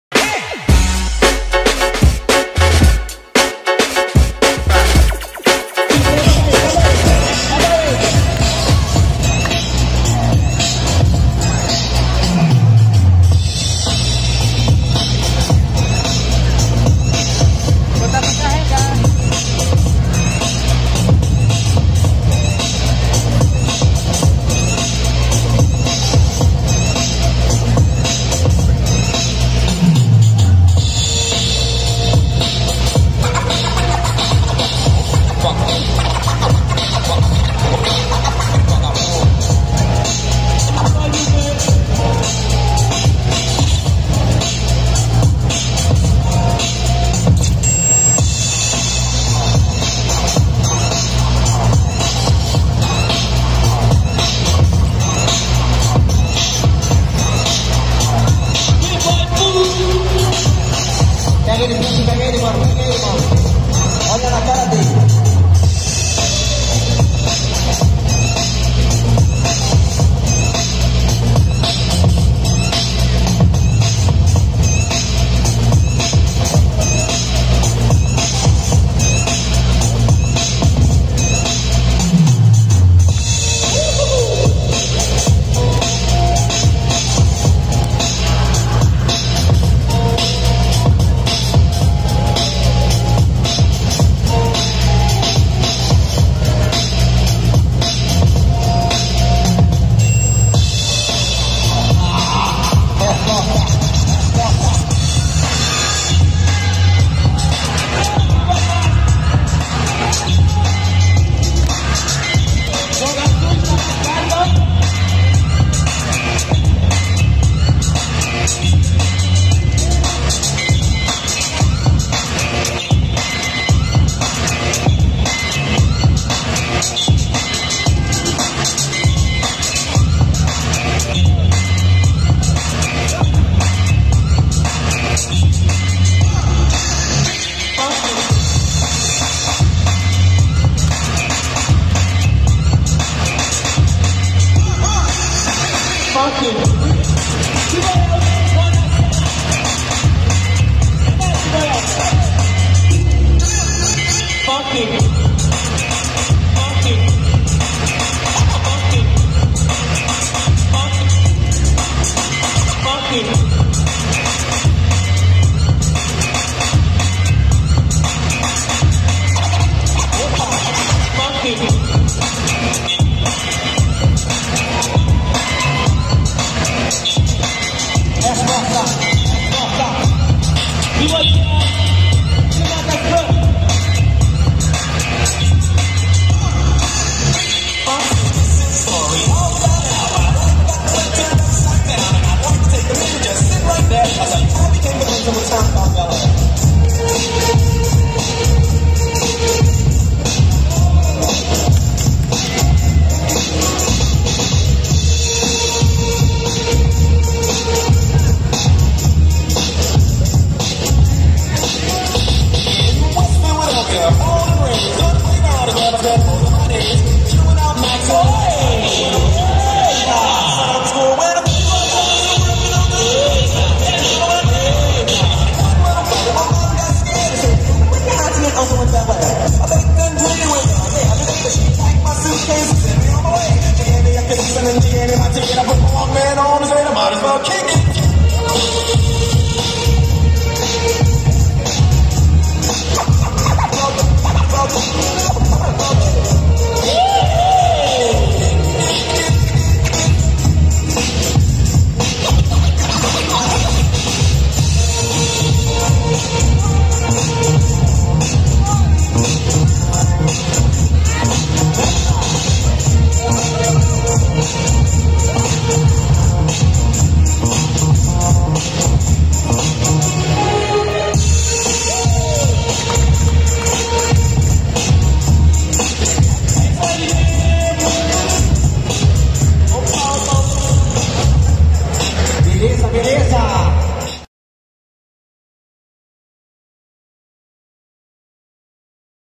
Lil B, Tiago & Kassio vs Pufe, Elton & Zoin | BREAKING | Batalha Del Monster 2022
Evento Batalha Del Monster realizado no dia 10 de abril de 2022 em Manaus, Amazonas.

Obrigado por está aqui!